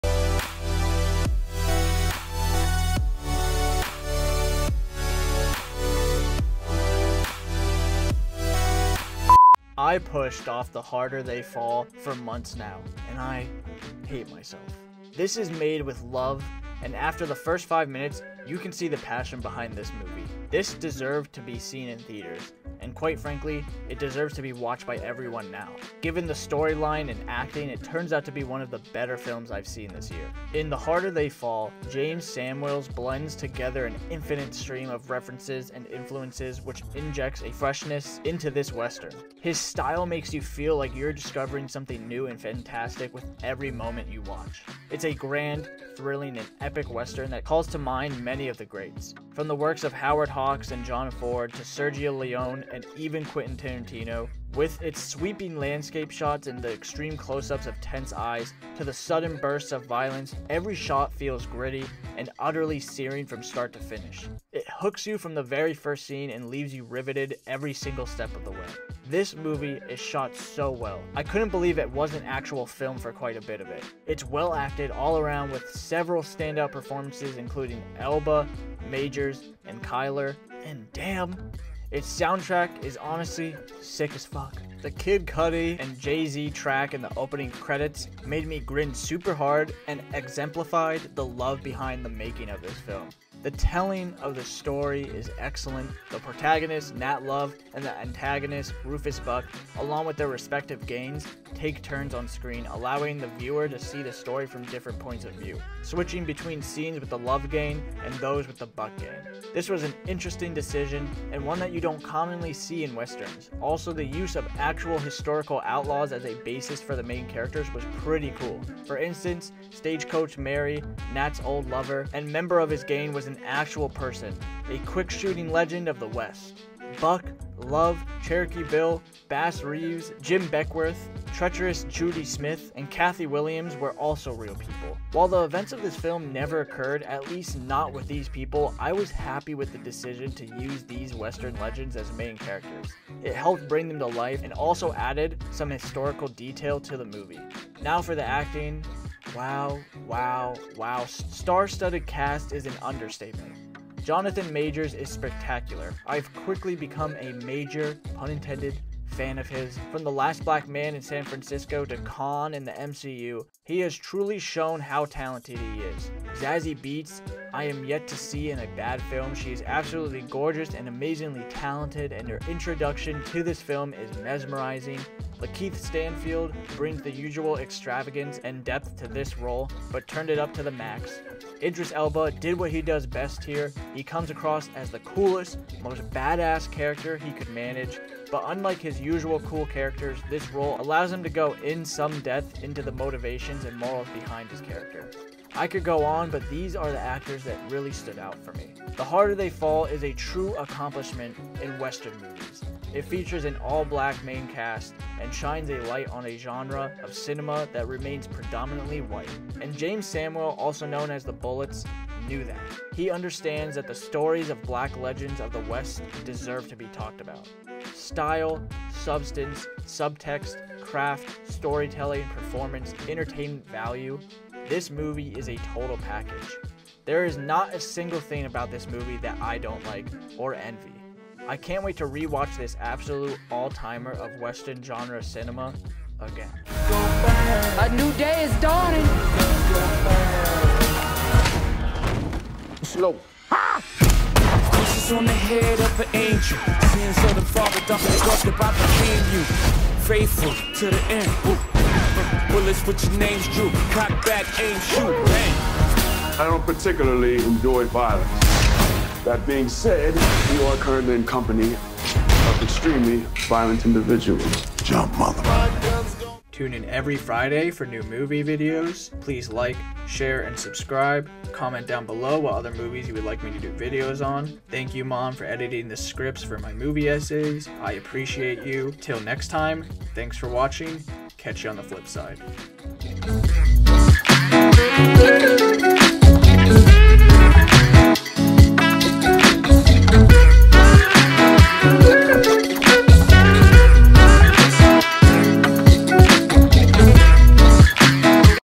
the harder they fall: style and soul (movie review)
Nat Love. Nuff said.
Letterboxd: @mrhonthy
Written & Edited by
Chris Honthy
Co-Written by
Christina Honthy

If you don't like the movie, then just don't watch my video :)